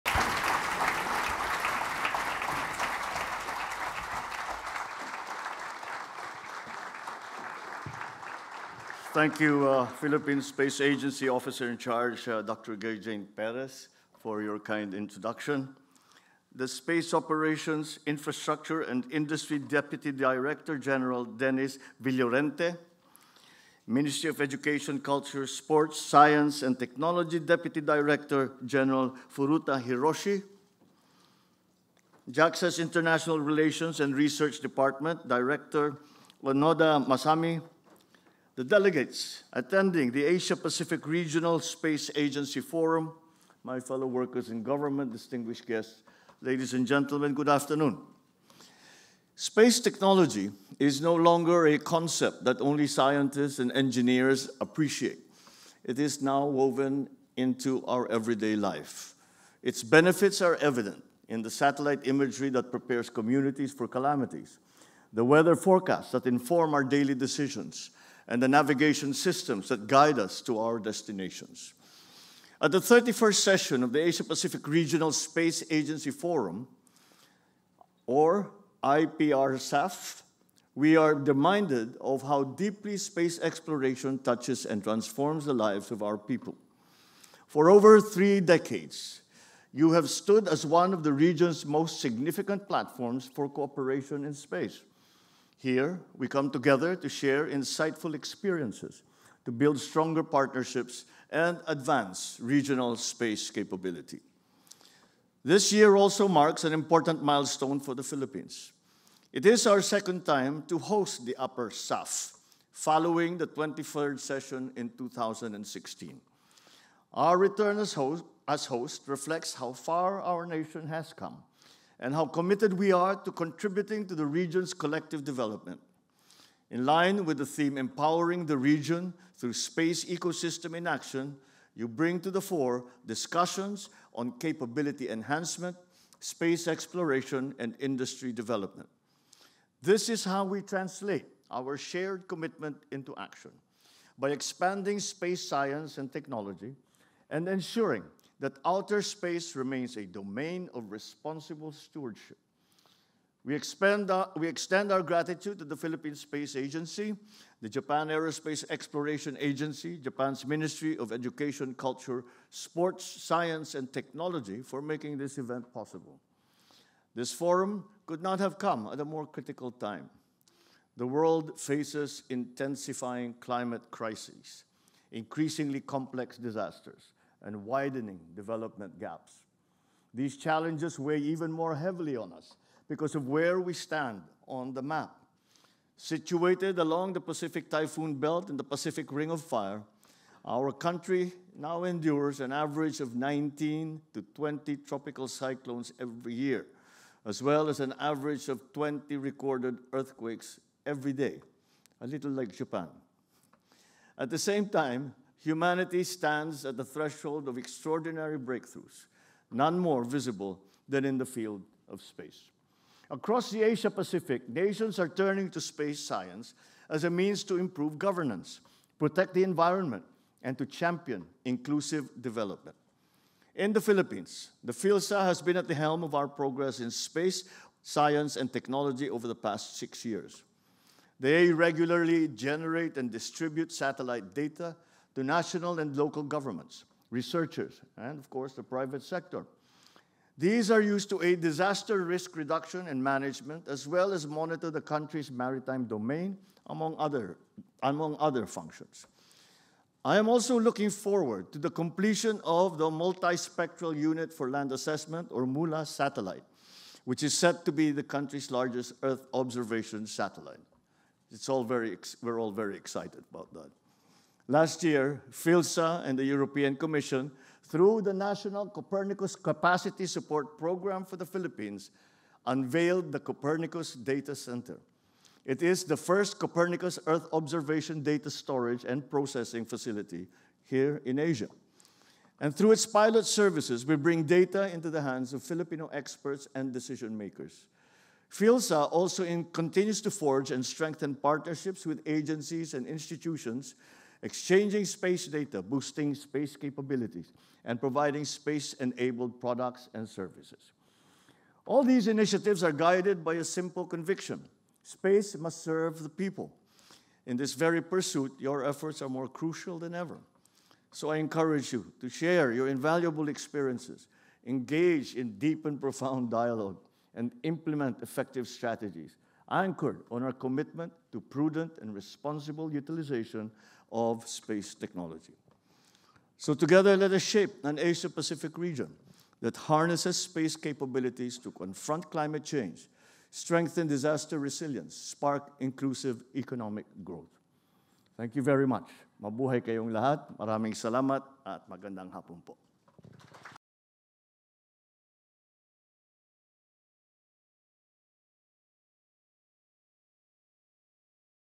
31st Session of the Asia-Pacific Regional Space Agency Forum
Shangri-La Mactan Cebu, Lapu-Lapu City

“Space technology is no longer a concept that only scientists and engineers appreciate — it is now woven into our everyday life,” stated President Ferdinand R. Marcos Jr., emphasizing space technology’s central role in governance, disaster management and development, during the country’s hosting of the 31st Session of the Asia-Pacific Regional Space Agency Forum in Shangri-La Mactan Cebu, Lapu-Lapu City.

Speaking before space leaders, scientists, researchers, students, astronauts and policymakers in Asia-Pacific, he highlighted the Forum as an essential platform for regional cooperation, experience-sharing and advancing space capabilities across the region.

With the Philippines hosting this year and carrying the theme ‘Empowering the Region through Space Ecosystems in Action,’ President Marcos Jr. emphasized the Philippines’ commitment to contributing to the region’s collective development.

According to the President, a major upcoming milestone is the completion of the Multispectral Unit for Land Assessment (MULA) satellite, which will become the Philippines’ largest Earth observation satellite.  The project highlights the country’s ambitions in comprehensive Earth observation and data-driven decision-making.

He also highlighted a major partnership announced last year between the Philippine Space Agency (PhilSA) and the European Commission (EC), through the Copernicus Capacity Support Action Programme, which established the Copernicus Data Centre — the first of its kind in Asia. The facility stores and processes Copernicus Earth Observation data, with pilot services designed to provide Filipino experts and decision-makers with access to it.

“All these initiatives are guided by a simple conviction: Space must serve the people. In this very pursuit, your efforts are more crucial than ever… So together, let us shape an Asia-Pacific region that harnesses space capabilities to confront climate change, strengthen disaster resilience, [and] spark inclusive economic growth,” President Marcos Jr. said in concluding his message.